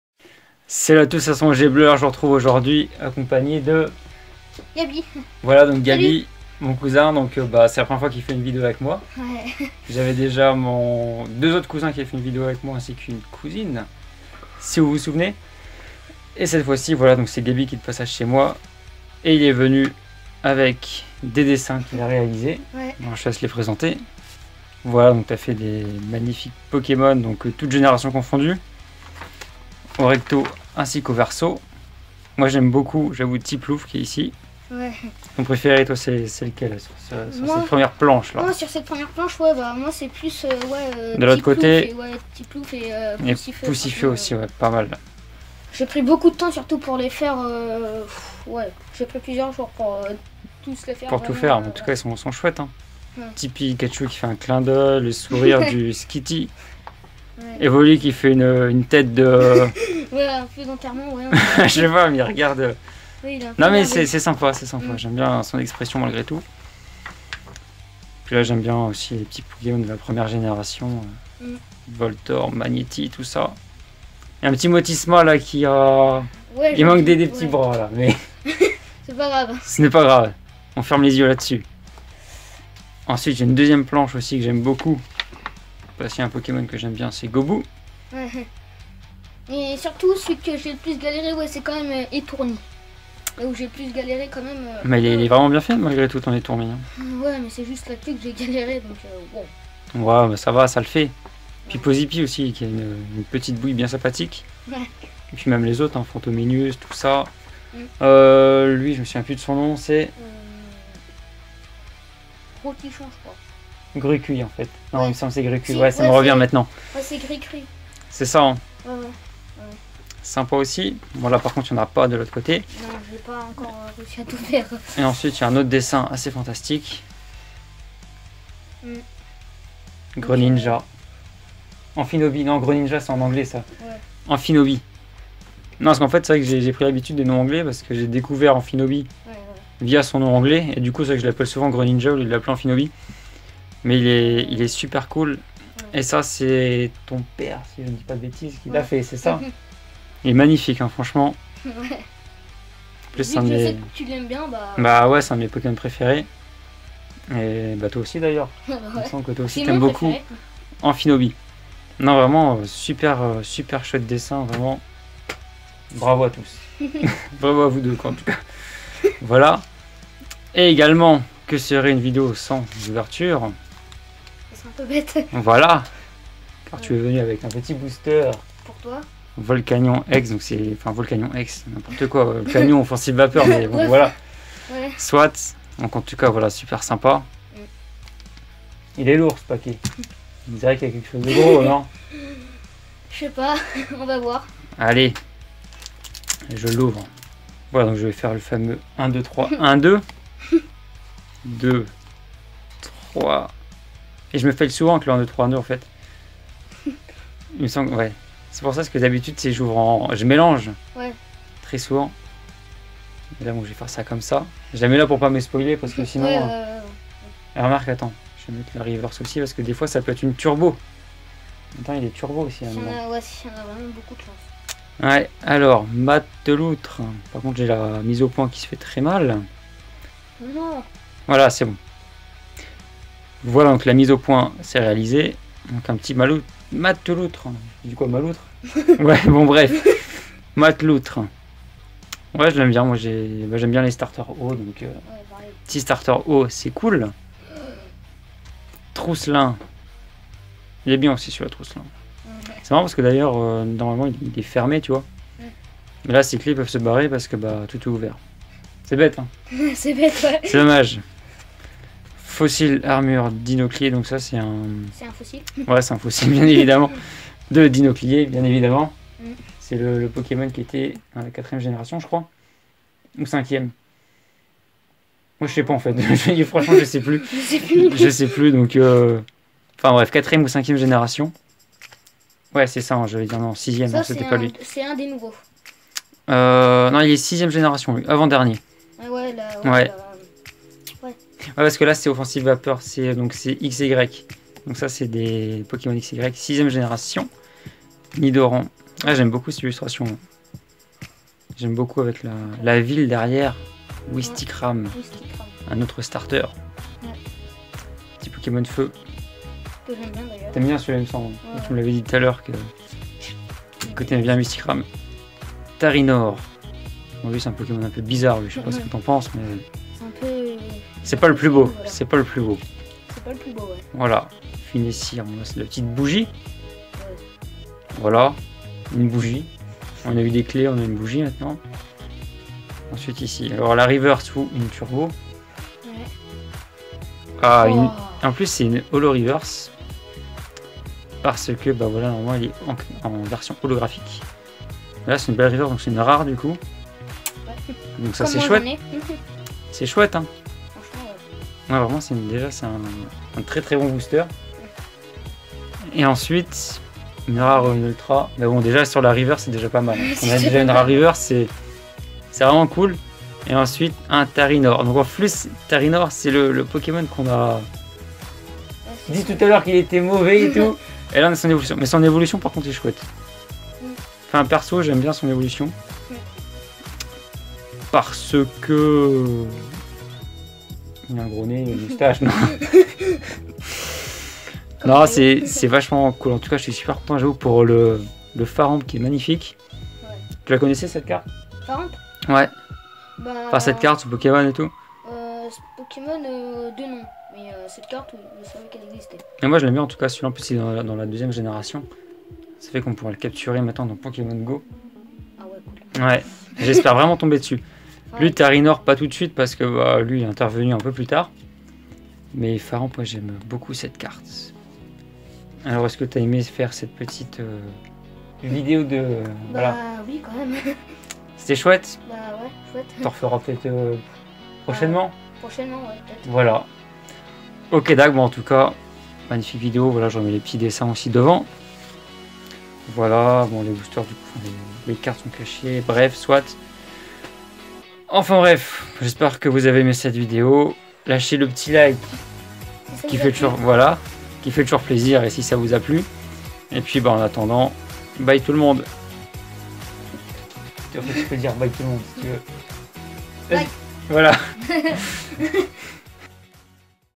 DESSIN AMPHINOBI + Ouverture Booster Pokémon Avec Mon Cousin!
[ DESSIN AMPHINOBI GRENINJA / ouverture de boosters Pokémon / booster pokémon / cartes pokémon / carte pokémon / Présentation Dessins / Offensive Vapeur ]

Ce week end mon cousin Gaby est venu me rendre visite et il m'a apporté de magnifiques dessins ainsi qu'un booster XY Offensive Vapeur qu'on a décidé d'ouvrir ensemble  en vidéo :)